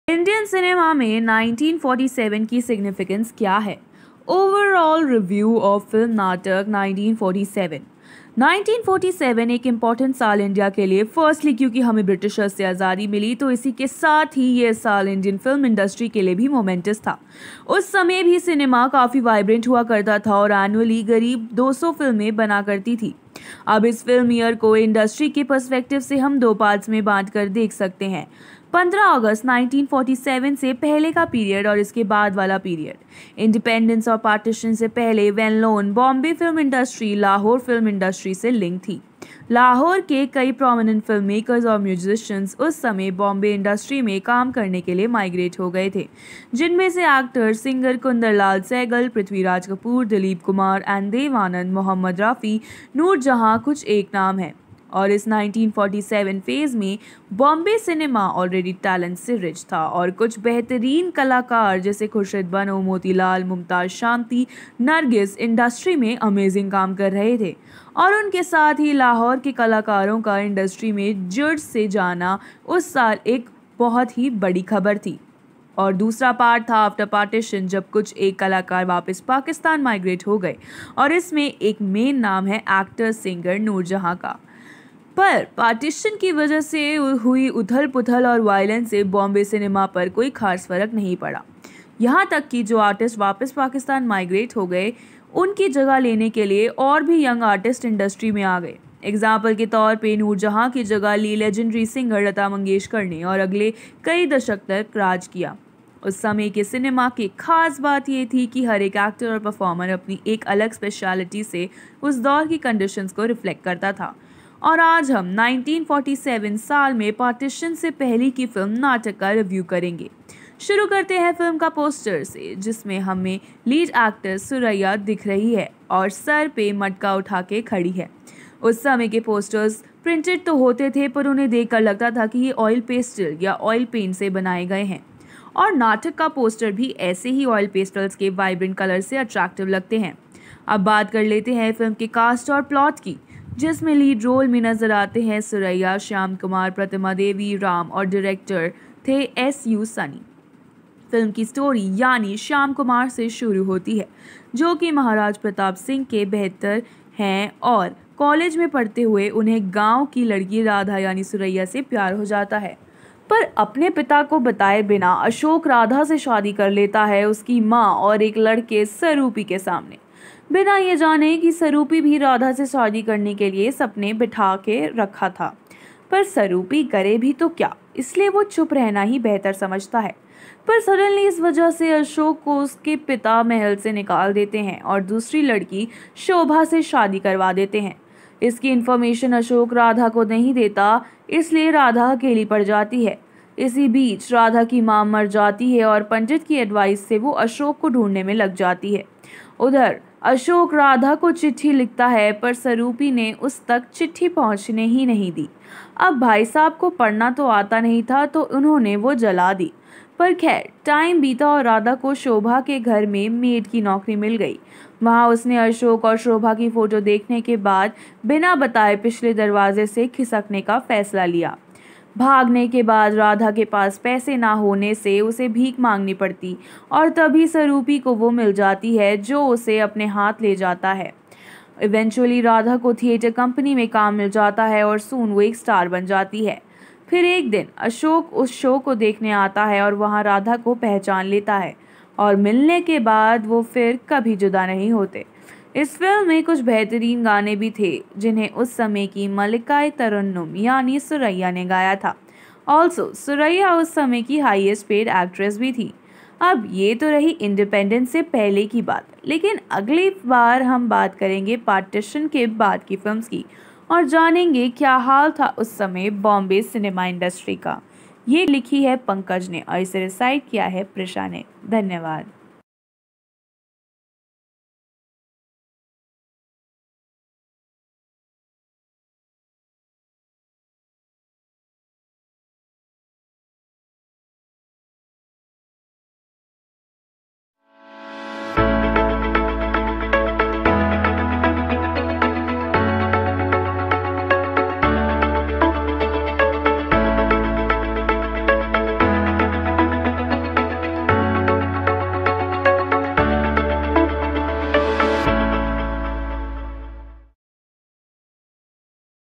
Natak (1947) | 51 Interesting Facts | S. U. Sunny | Surayya, Amar, Sufia Bano
Natak is a social film released in 1947. The film is directed by S. U. Sunny under the banner of Kardar Productions. The cast includes Surayya, Amar, Sufia Bano, Sheyam Kumar, M. D. Kanwar, Ameer Ali and Nazeer Bedi.

Naatak is a Bollywood film. It was released in 1947. It has been released for free on YouTube. This film is known to be one of the earliest Indian film to be partly couloured.

Initial release: 1947
Director: S.U. Sunny
Music composed by: Naushad

Writer : Pankaj
Anchor : Prisha
Editor:Nalla Saurav kumar Archives